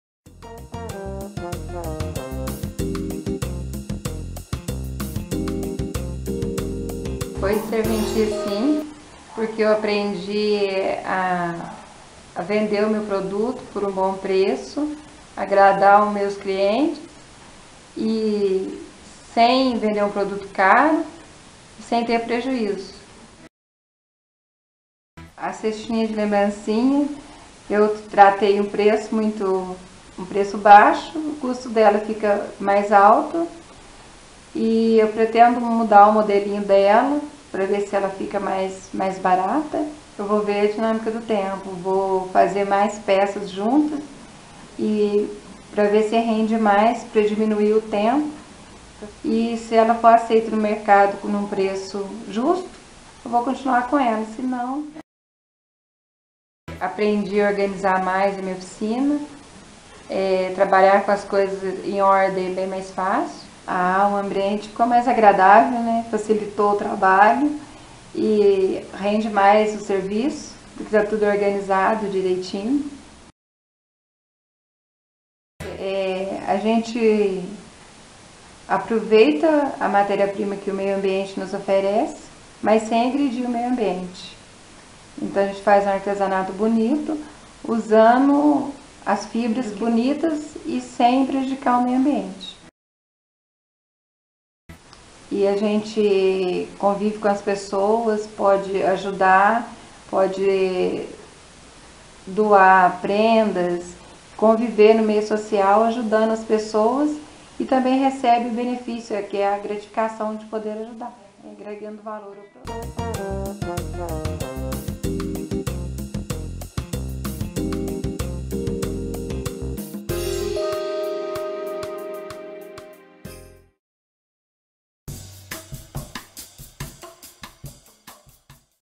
Fique Ligado Certificação Produção Artesanal Artesã Lucília
Entrevista com a artesã Lucília sobre a participação no Projeto de Fortalecimento da Indústria Artesanal - Por meio do Instituto Centro de Capacitação e Apoio ao Pequeno Empreendedor (ICCAPE),SESI e a Federação das Indústrias do Estado de Minas Gerais -- FIEMG.